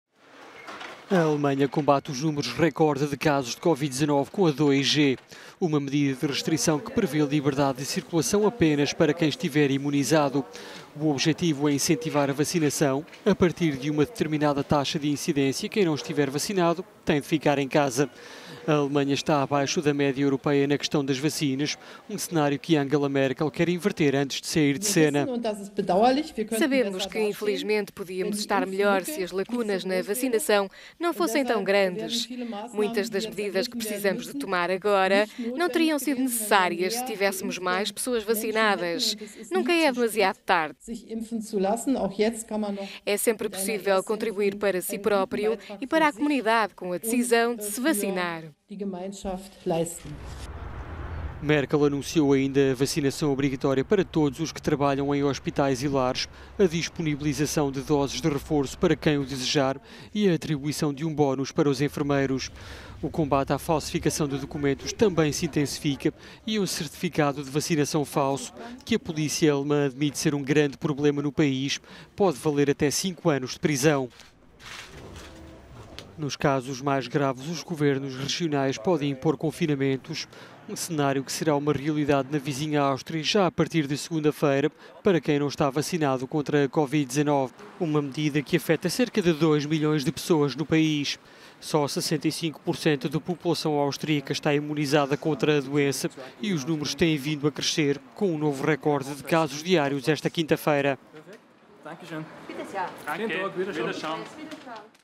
Alemanha e Áustria apertam medidas de restrição
Ambos os países estabeleceram um novo recorde de casos diários de covid-19 esta quinta-feira